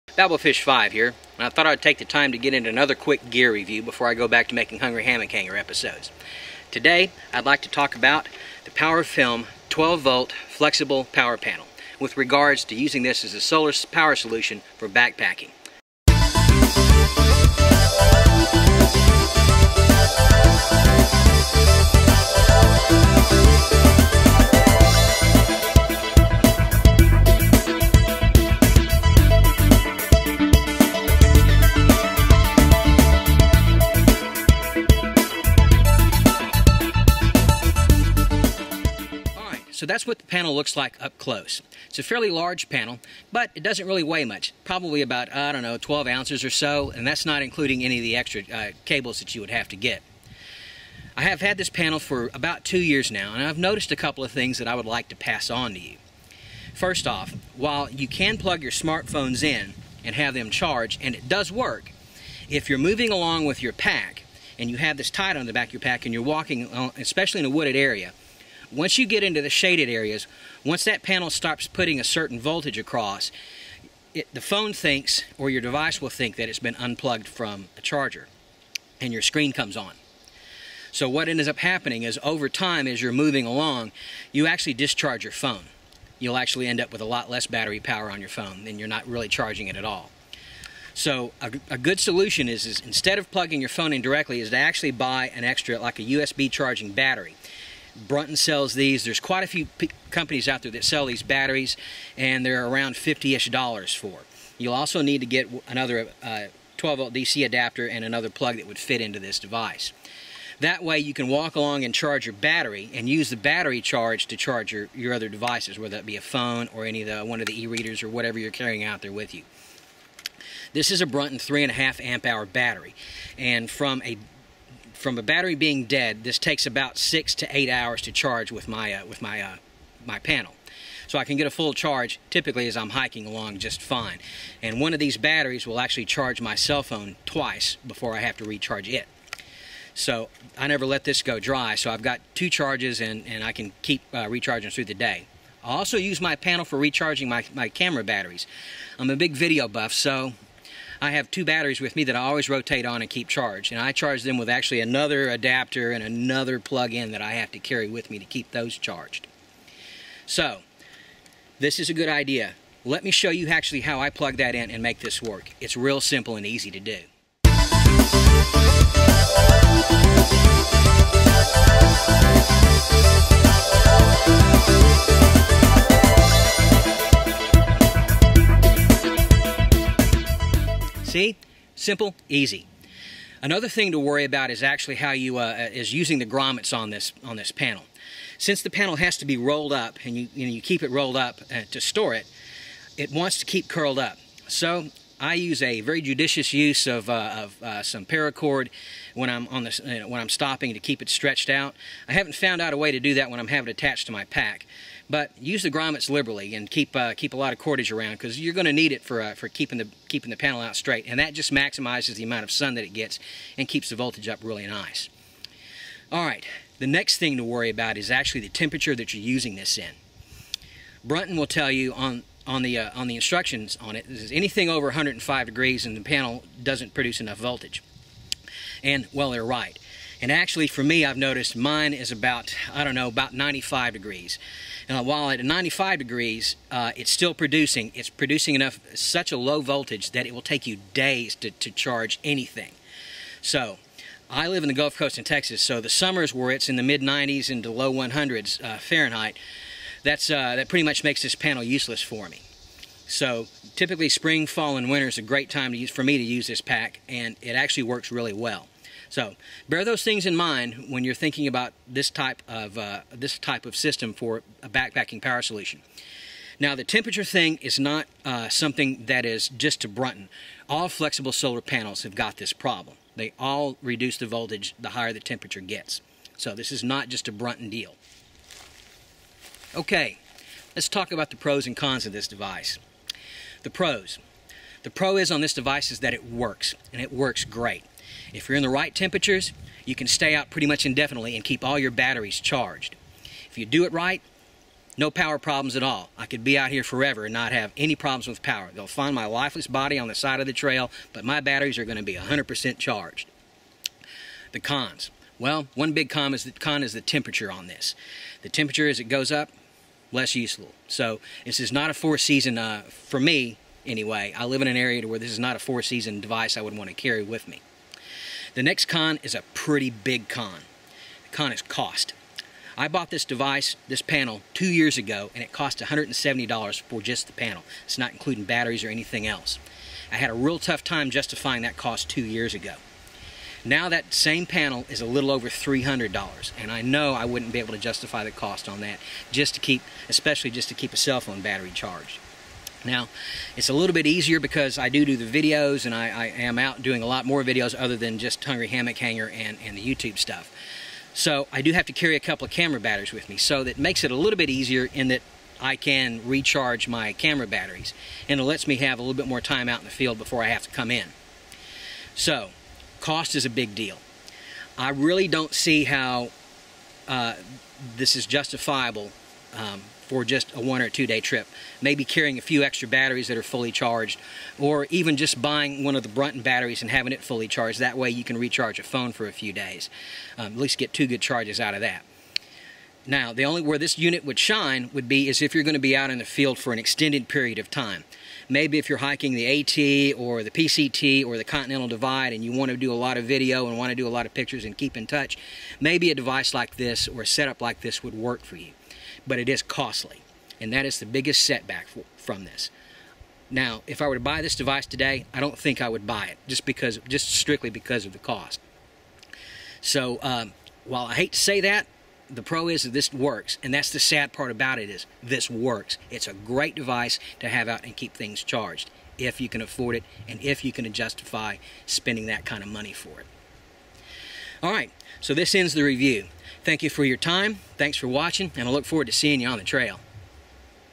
Backpacking Solar Battery Charger PowerFilm Flexible Solar Panel Review
A review of a 12 Volt 14 Watt PowerFilm Rollable Solar Panel used as a backpacking power solution. I've used this setup successfully for a couple of years and have some strong opinions about it so, I get a bit long winded about it.